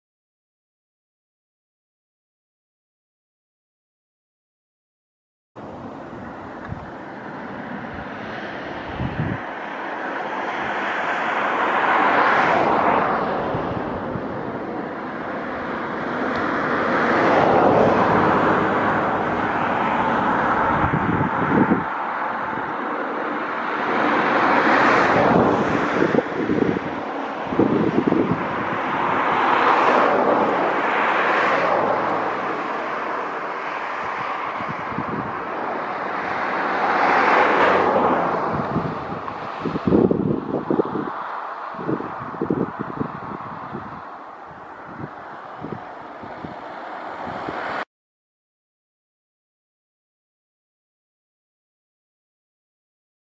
HB-JZX easyJet Switzerland Airbus A320-214 | Canon SX60 HS
HB-JZX easyJet Switzerland Airbus A320-214 Landing RWY 12 TLV Ben Gurion Airport 15/10/2014 Canon PowerShot SX 60 HS